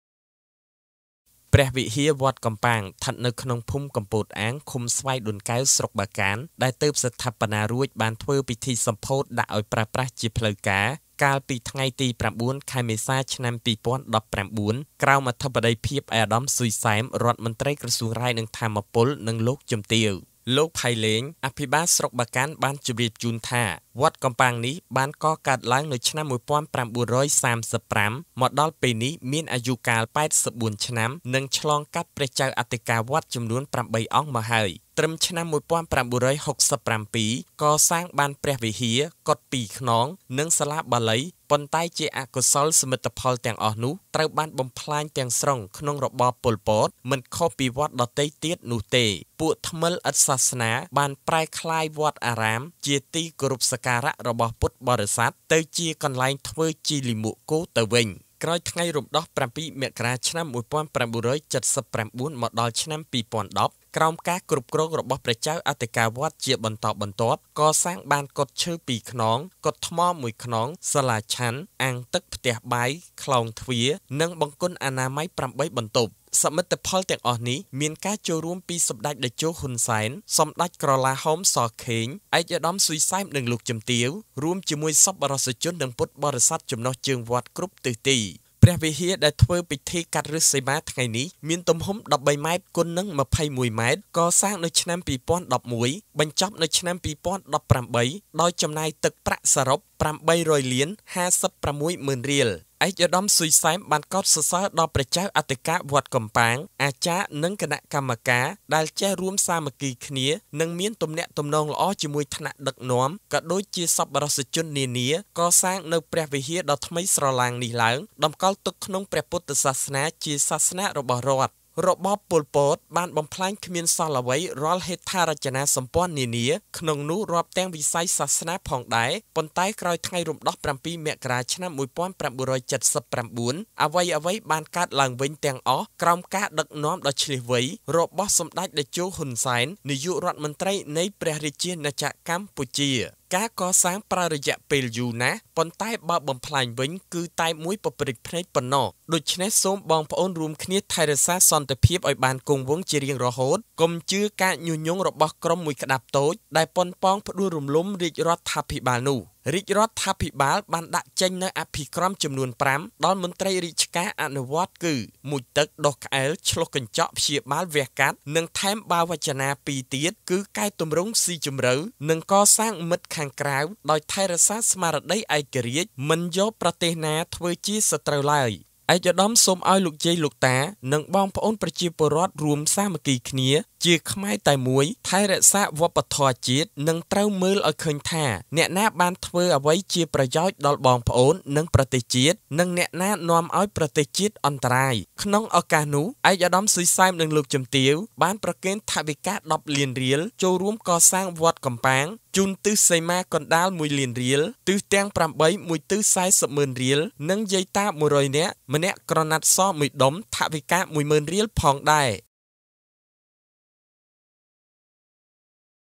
ឯកឧត្តមស៊ុយ សែមនិងលោកជំទាវសម្ភោធព្រះវិហារវត្តកំប៉ាង
ខេត្តពោធិ៍សាត់៖ ព្រះវិហារវត្តកំប៉ាងស្ថិតក្នុងភូមិគម្ពោធអាងឃុំស្វាយដូនកែវស្រុកបាកានដែលទើបស្ថាបនារួច បានធ្វើពិធីសម្ភោធដាក់ឲ្យប្រើប្រាស់ជាផ្លូវការ កាលពីថ្ងៃទី០៩ខែមេសាឆ្នាំ២០១៩ក្រោមអធិបតីភាពឯកឧត្តម ស៊ុយ សែម រដ្ឋមន្ត្រីក្រសួងរ៉ែនិងថាមពលនិងលោកជំទាវ។